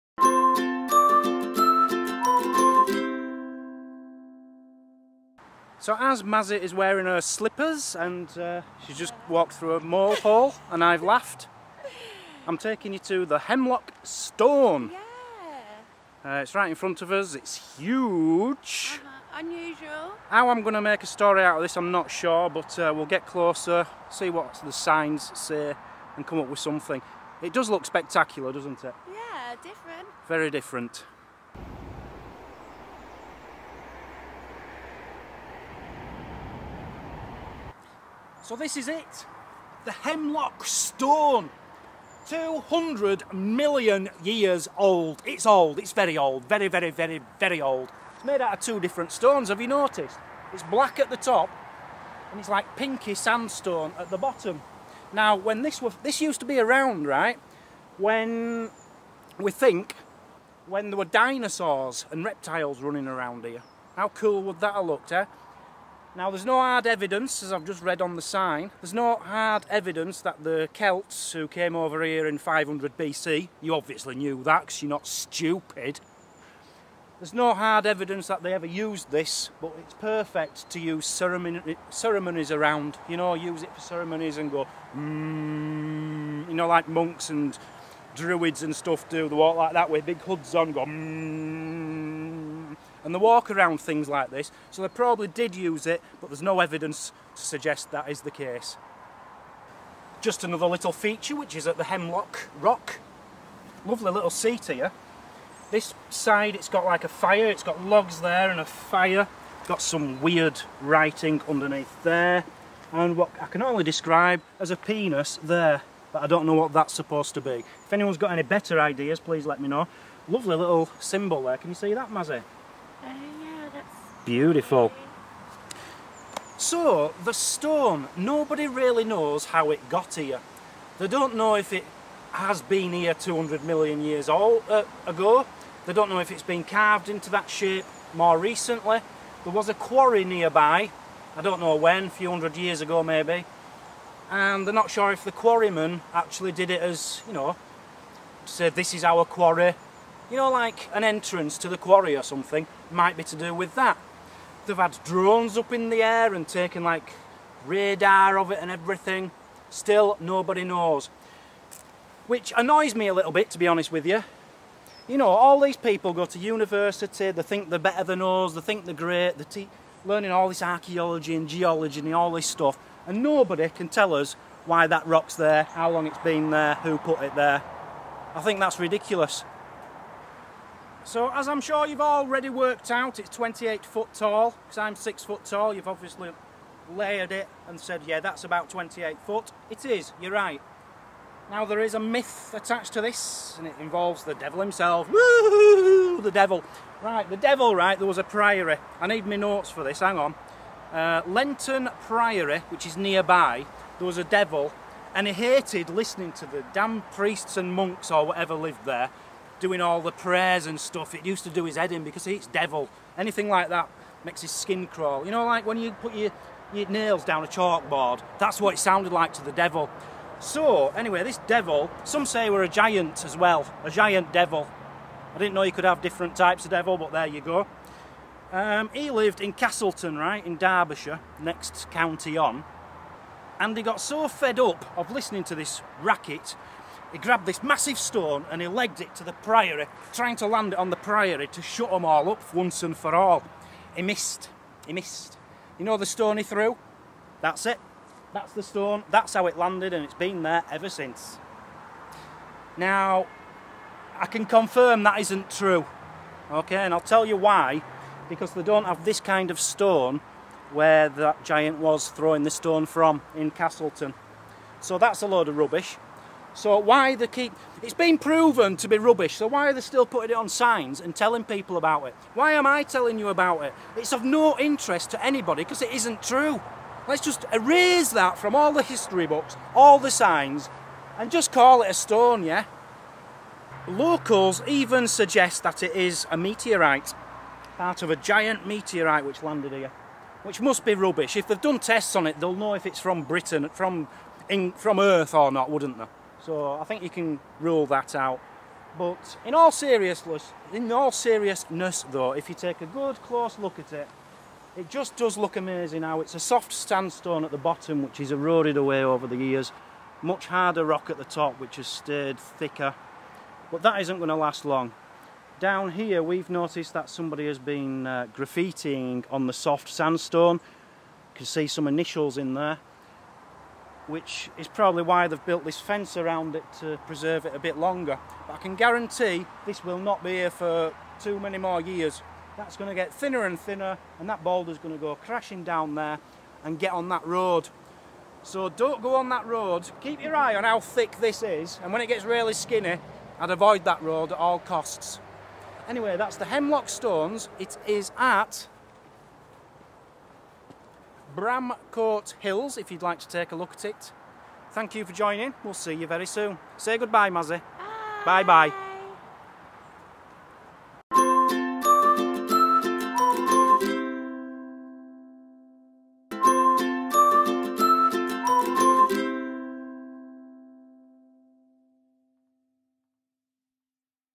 Did The Devil Throw This Stone?
More travels take us to the Hemlock Stone, a giant stone thrown by a giant devil many years ago. Can we really believe the story? It really does look out of place here and how it got here we may never know.

Any mail can be sent to our Travel Trolls TV address:
Travel Trolls TV
Bury St Edmunds
IP33 9LS